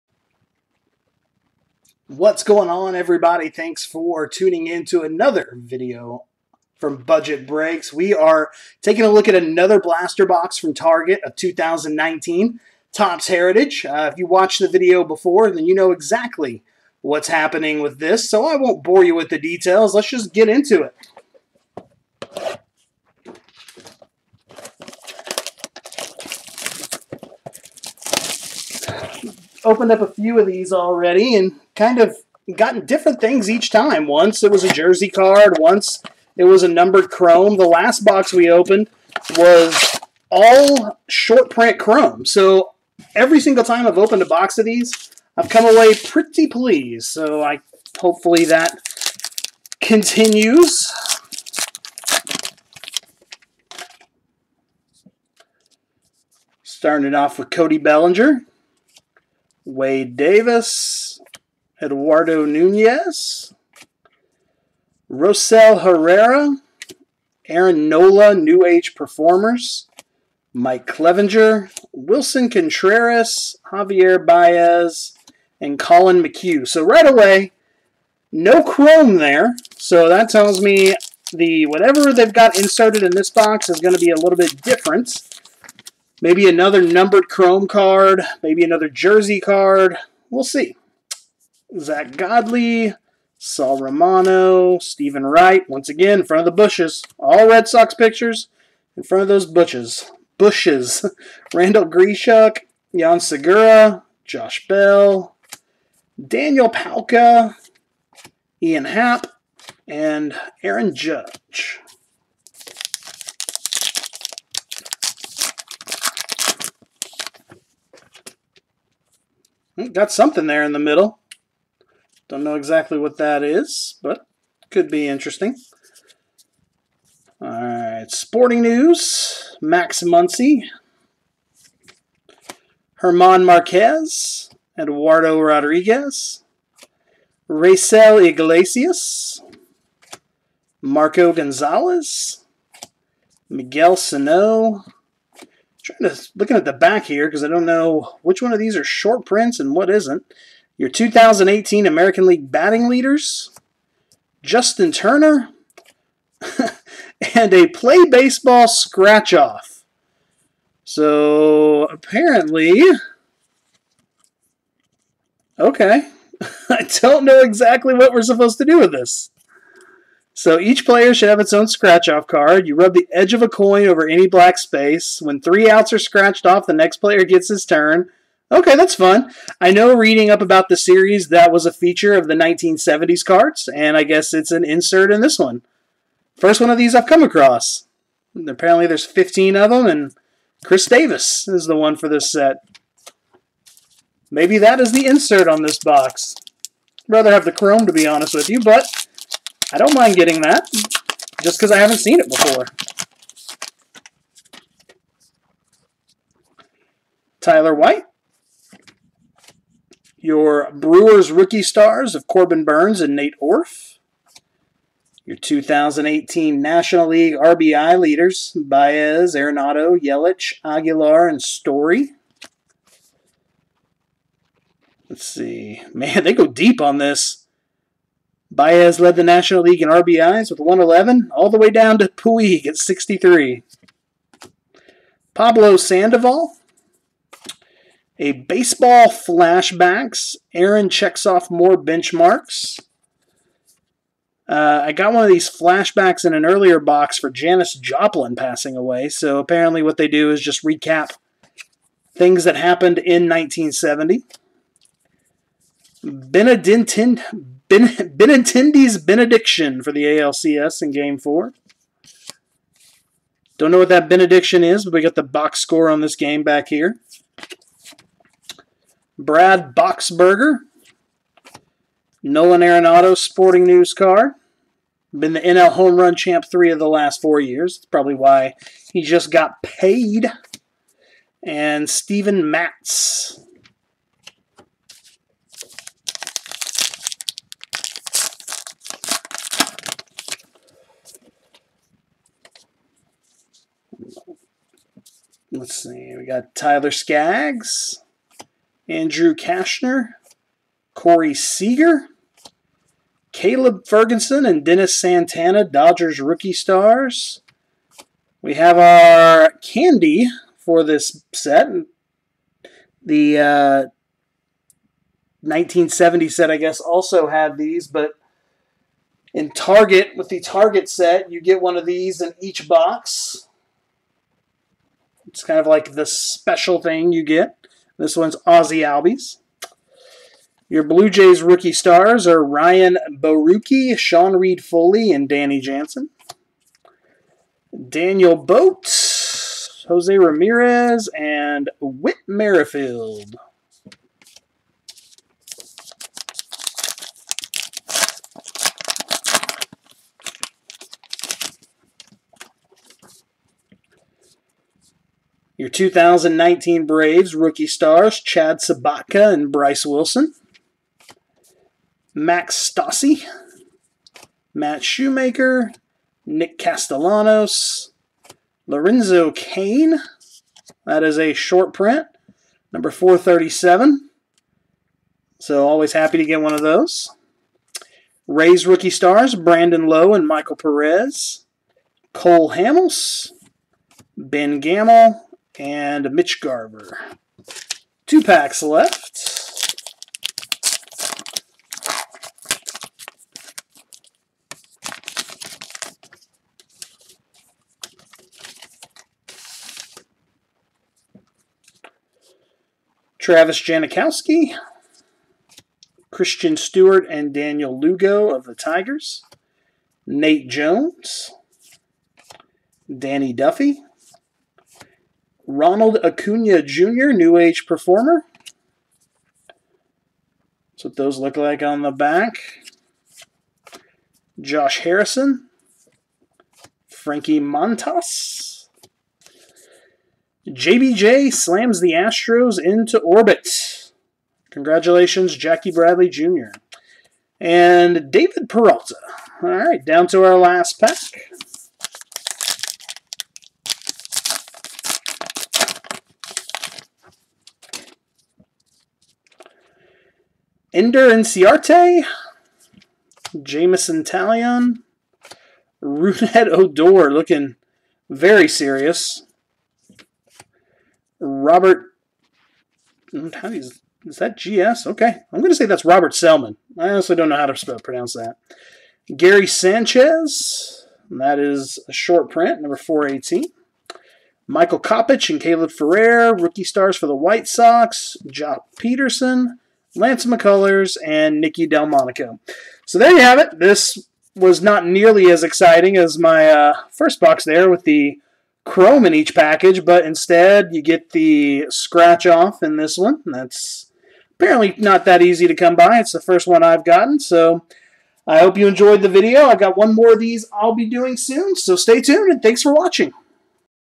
2019 Topps Heritage Baseball Blaster Box Break!
We take a look at 2019 Topps Heritage.